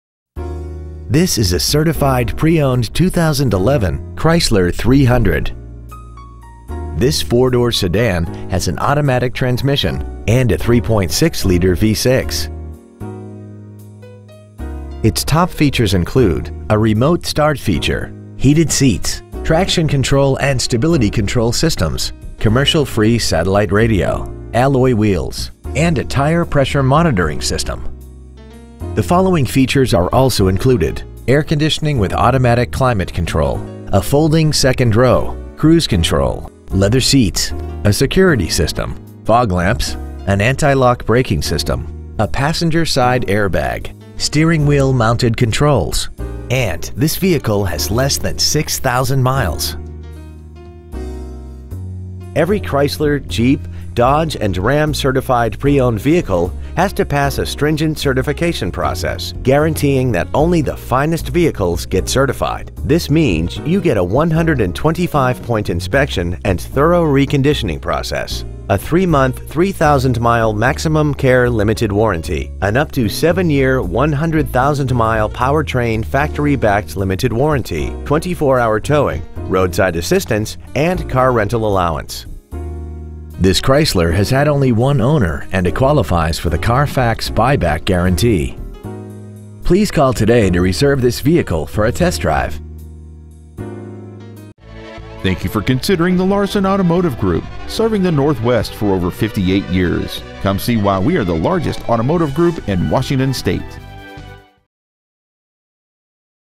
Used 2011 Chrysler 300 Puyallup WA
Year : 2011

 Make : Chrysler

 Model : 300

 Engine : 3.6L V6 SOHC 24V

 Trans . : Automatic

 Exterior : Black

 Miles : 5,474

 Interior : Black

 Stock : 7438

 2001 N Meridian

 Preowned 2011 Chrysler 300 Puyallup WA